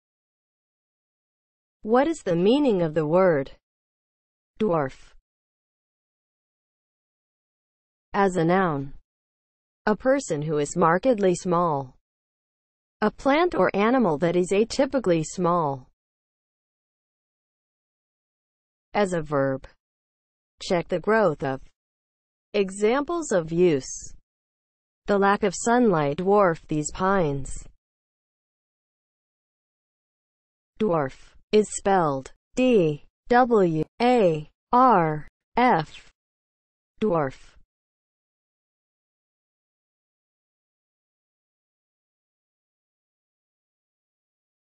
What is the meaning of the word DWARF?
Definition, examples of use and spelling of the word DWARF.

(00:00) Intro
(00:07) As a noun
(00:17) As a verb
(00:28) Spelling

As a noun (00:07):
• a person who is markedly small
• a plant or animal that is atypically small

As a verb (00:17):
• check the growth of

Examples:
• "the lack of sunlight dwarfed these pines"